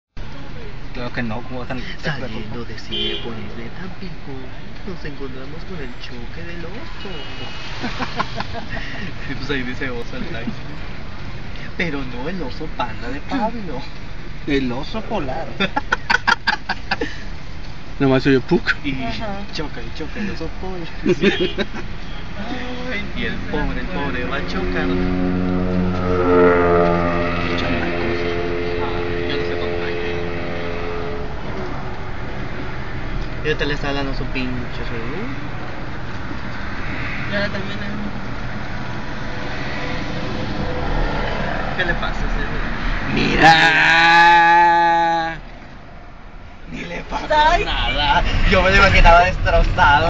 Accidente - cinepolis - tampico - paparazzi
al salir de cinepolis tampico
es un problema
vean por que...

todos se avientan
falta mas vigilancia..!!